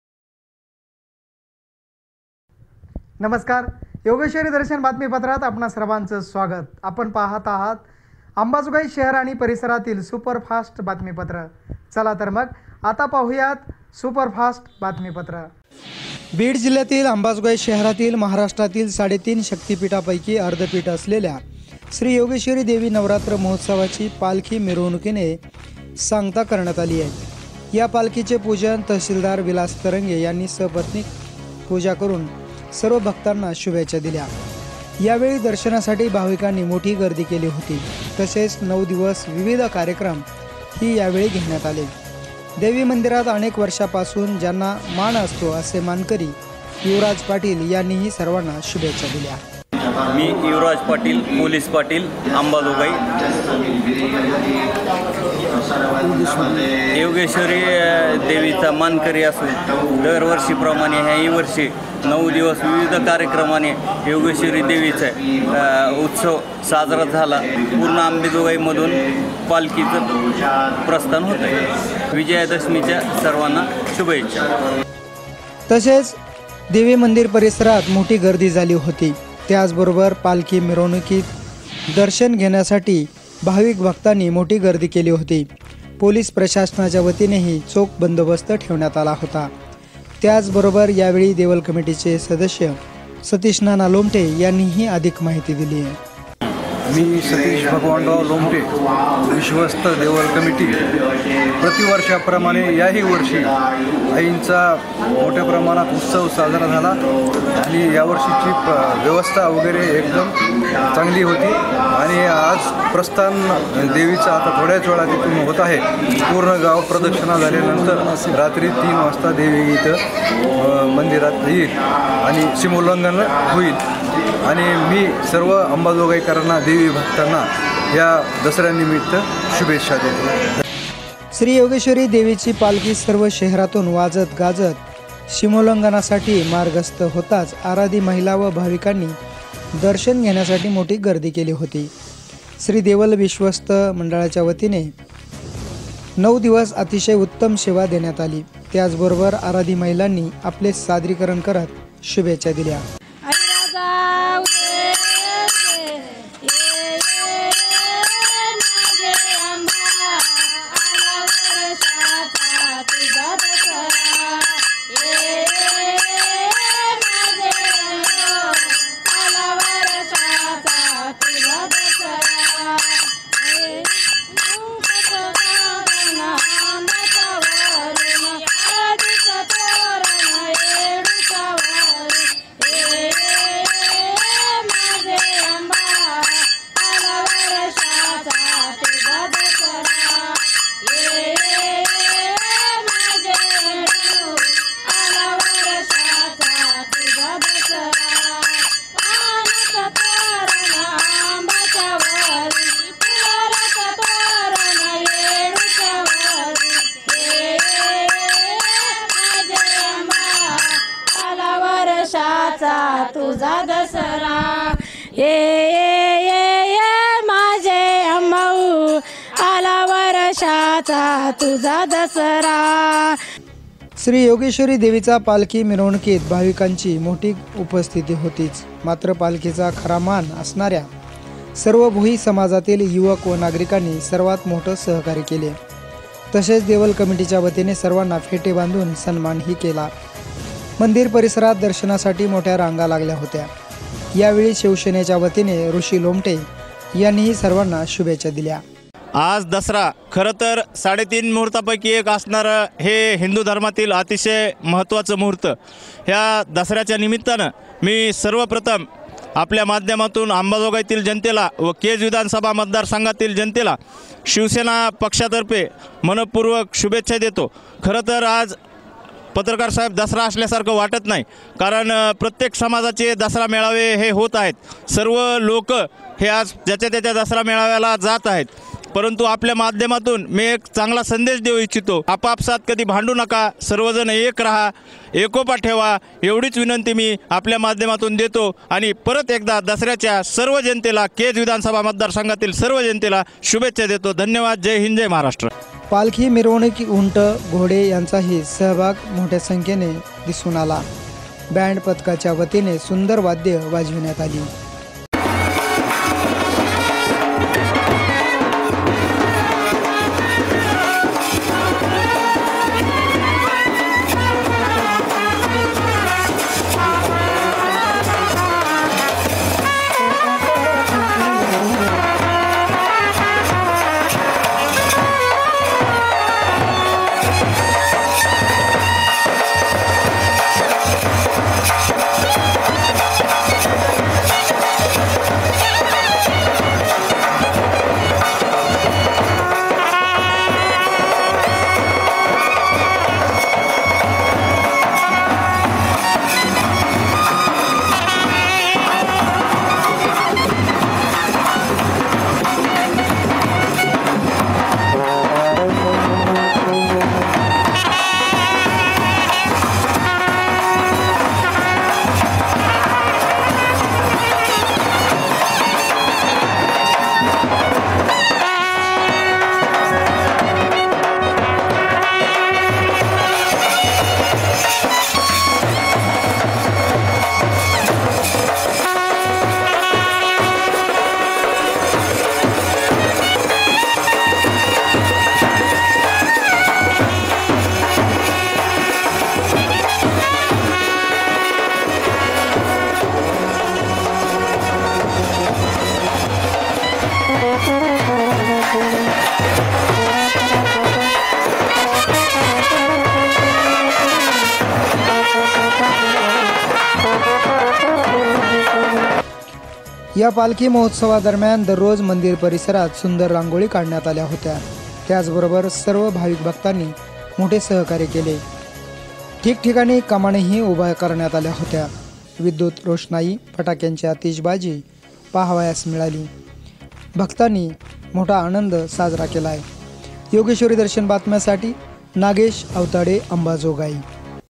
श्री योगेश्वरी देवीचा नवरात्र महोत्सवाची पालखी मिरवणुकीने सांगता
yogeshwari palkhi